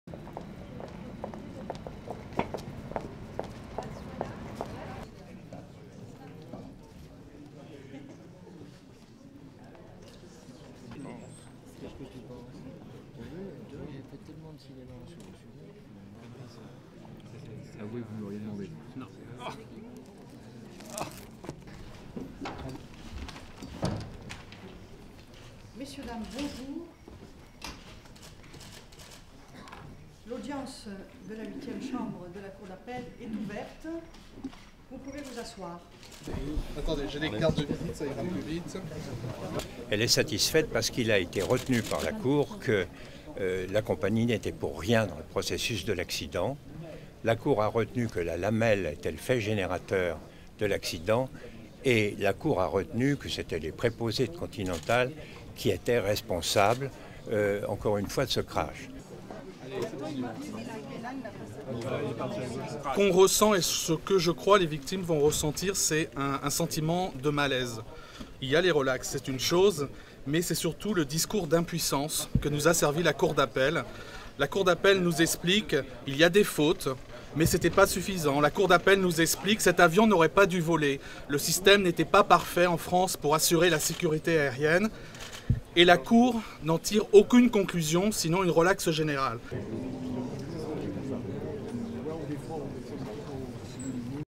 French court overturns Concorde crash conviction
1. Wide exterior of Court building in Versailles
2. Wide interior of the court
3. Mid of Air France legal team (Fernand Garnault, Air France lawyer on the right)
4. Olivier Metzner, lawyer for Continental Airlines
5. Wide of the judge Michele Luga entering and opening the session
6. Wide of media
7. SOUNDBITE: (French) Fernand Garnault, Air France lawyer:
"(Air France) is satisfied because the court decided that Air France didn't have any role in the accident (of Concorde) and that Continental is supposedly responsible for the crash."
8. Cutaway camera
9. SOUNDBITE (French) Stephane Gicquel, head of a group of victims' families:
"What we feel and what I think that (families of the) victims will feel is a sense of powerlessness. There is the discharge (of Continental Airlines), that is one thing but it is the discourse of helplessness that was served to us by the Court of appeal. The court of appeal explained to us that there are some faults, but that is not enough; the court explained to us that that plane was not supposed to fly, that the system was not perfect in France to assure the airline security and the court doesn't conclude anything but a general discharge."
10. Wide of courtroom interior
STORYLINE:
A French appeals court overturned manslaughter convictions against Continental Airlines and a mechanic for the July 2000 crash of an Air France Concorde that killed 113 people, ruling on Thursday that their mistakes did not make them legally responsible for the deaths.
The crash hastened the end for the already-faltering supersonic Concorde, synonymous with high-tech luxury but a commercial failure.
The programme, jointly operated by Air France and British Airways, was taken out of service in 2003.
In the accident, which occurred on July 25, 2000, the jet crashed into a hotel near Paris' Charles de Gaulle airport soon after taking off, killing all 109 people aboard and four on the ground.
Most of the victims were Germans heading to a cruise in the Caribbean.
A mistake made weeks earlier and thousands of miles away by a Continental mechanic in Houston played a crucial role in the crash, the court found.
According to the original ruling, the mechanic fitted the wrong metal strip on a Continental DC-10.
The piece ultimately fell off on the runway in Paris, puncturing the Concorde's tyre.
The burst tyre sent bits of rubber into the fuel tanks, which started the fire that brought down the plane.
But the Concorde's design left it vulnerable to shock, according to judicial investigators who said officials had known about the problem for more than 20 years.
The lower court ruled that though French officials had missed opportunities to improve the Concorde over the years, they could "be accused of no serious misconduct."
Stephane Gicquel, head of a group of victims' families, said Thursday's ruling left them with "a sense of powerlessness."
"The court explained to us that that plane was not supposed to fly, that the system was not perfect in France to assure the airline security and the court doesn't conclude anything but a general discharge," Gicquel said.
A French court initially convicted Continental Airlines Inc. and the mechanic in 2010 for the crash, and imposed about 2 (m) million Euros (2.7 million US Dollars) in damages and fines on the carrier.
Parties including Air France and Continental compensated the families of most victims years ago, so financial claims were not the trial's focus, the main goal was to assign responsibility.